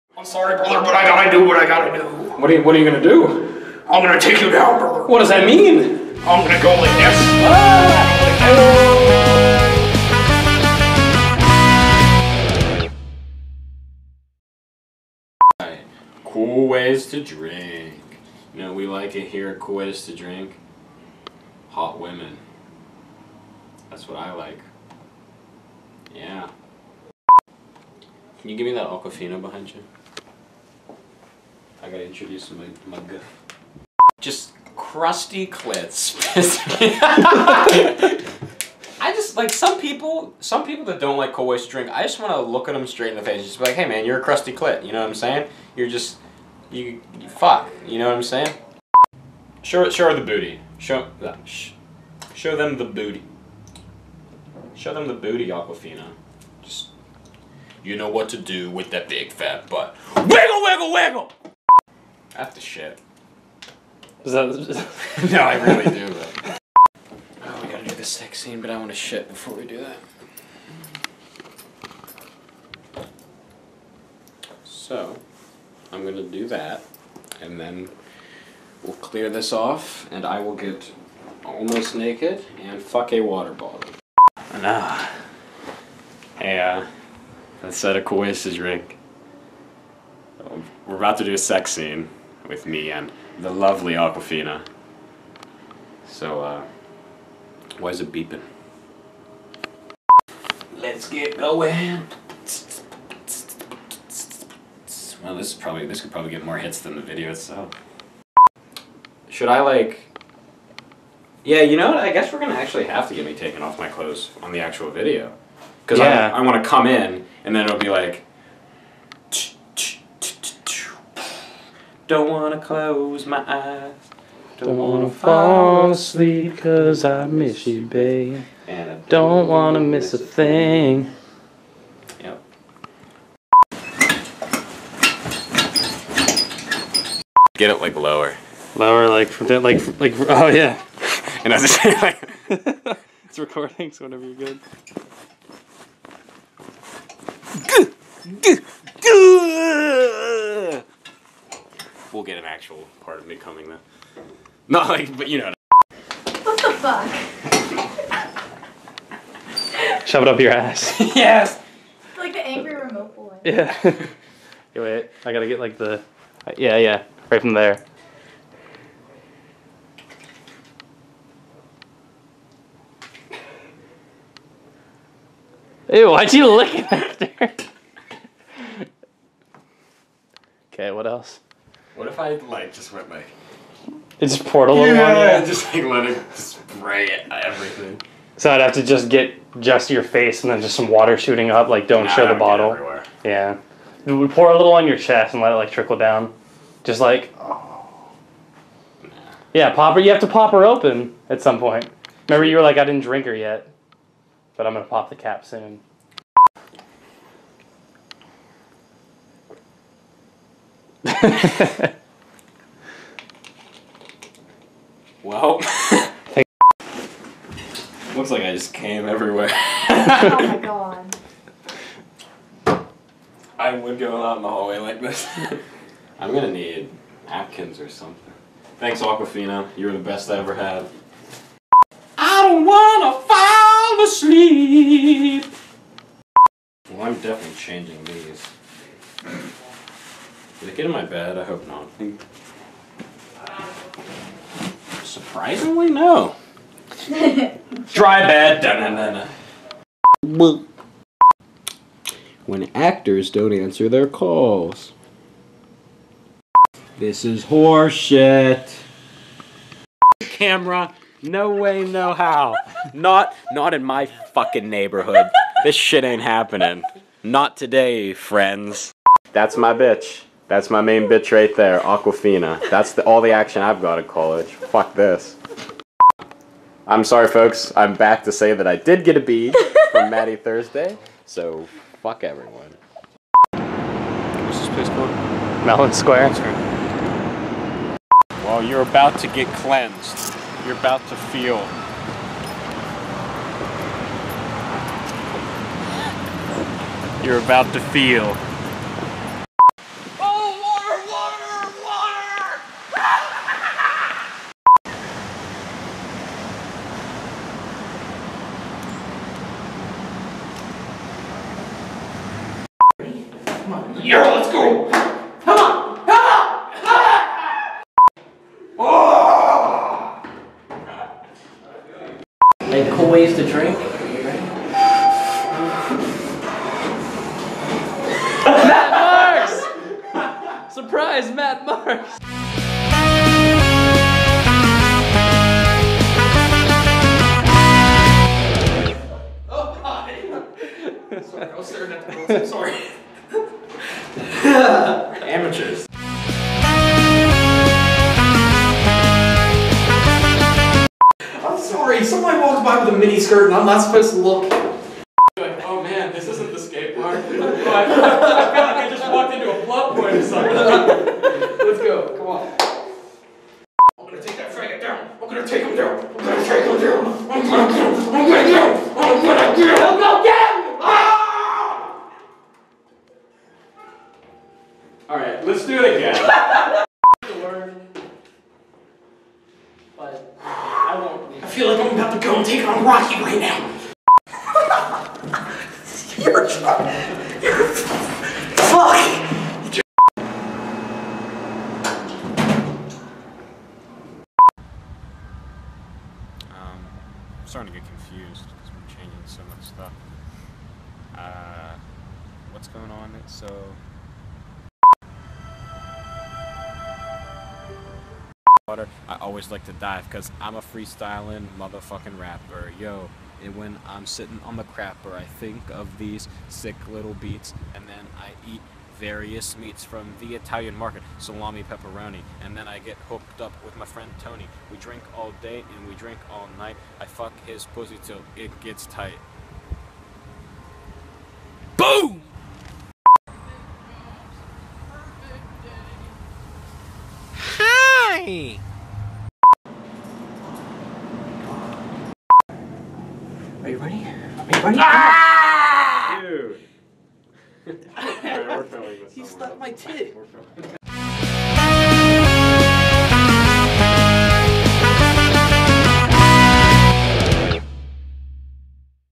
CWTD Season 1 Bloopers Episodes 2 & 3
Outtakes and bloopers for episodes 2 and 3 of Cool Ways to Drink.